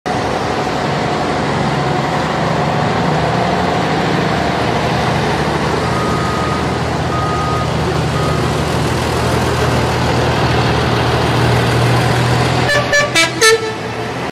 Long-Distance Classic Coach - Karosa LC 736
Czech long-distance classic coach "Karosa LC 736" spotted at the 6th european meeting of historic buses in Speyer (Germany).